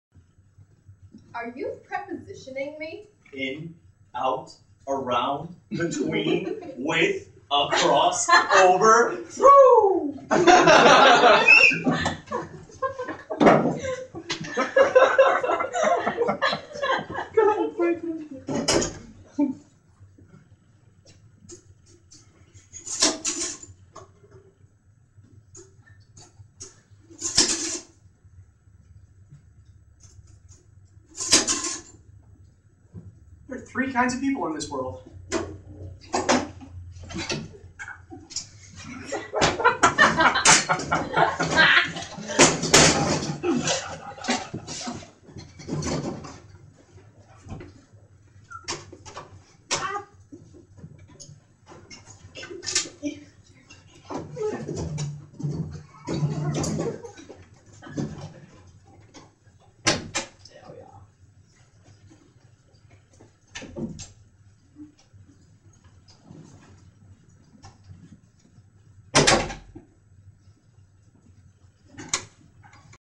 Like a Fox 11: Are You Prepositioning Me? / Adding Machine Gag #2
We prove just how nerdy we really are. Then Nick states a simple fact.

As with all these clips, the video was recorded on an old Hi8 camcorder without mics, so the audio/visual quality is poor.